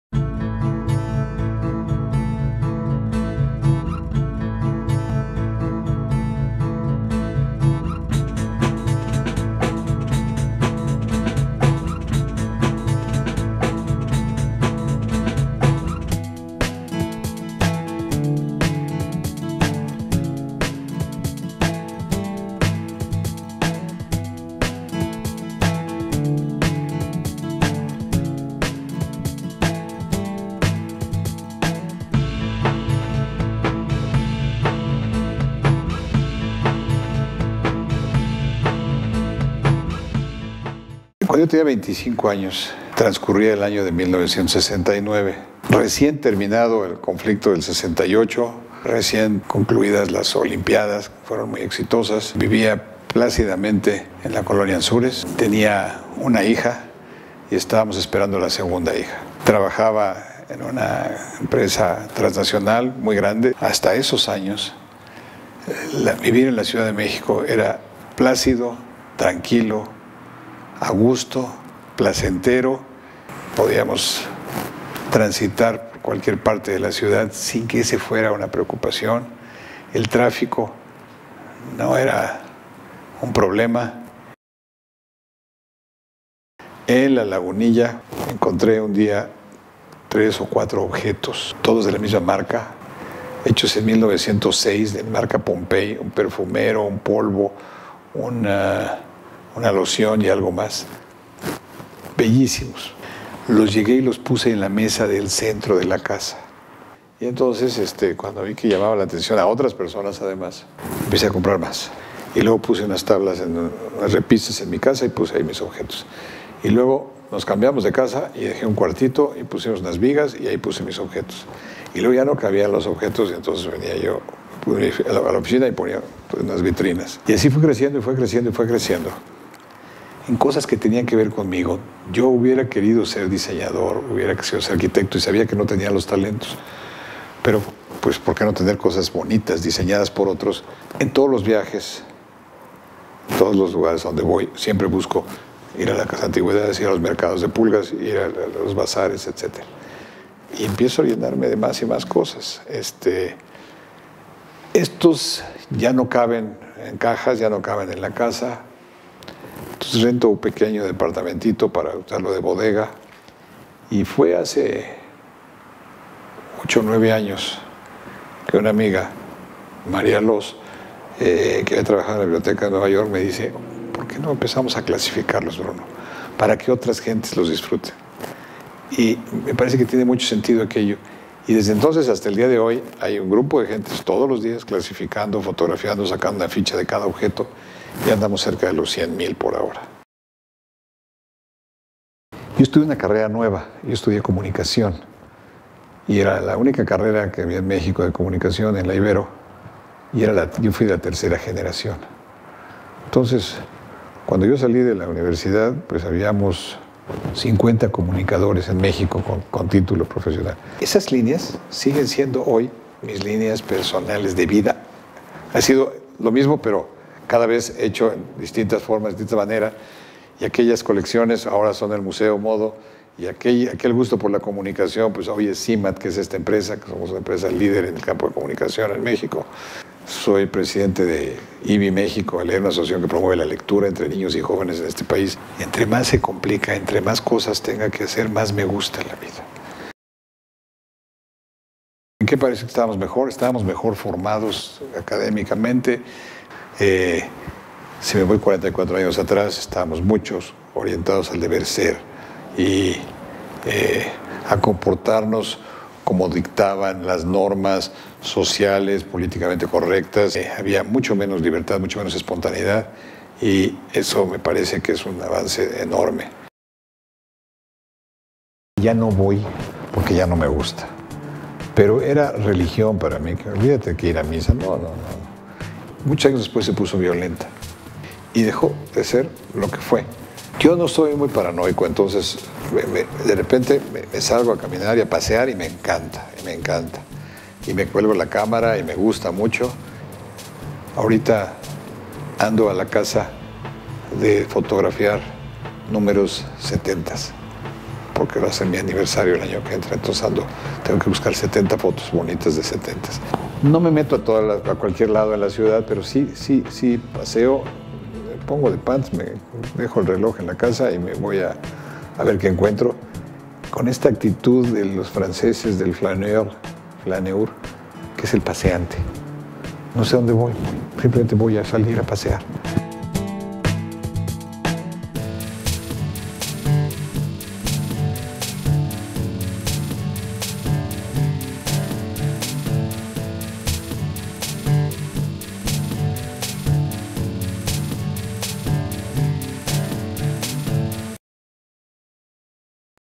Forbes México: El Coleccionista
Cuatro artículos de tocador de los inicios del siglo XX que adquirió en el mercado de La Lagunilla por 15 pesos, detonaron en Bruno Newman la idea de coleccionar tantas cosas como fuese posible. Hoy el empresario tiene cerca de 100,000 antigüedades y es el fundador del Museo del Objeto del Objeto.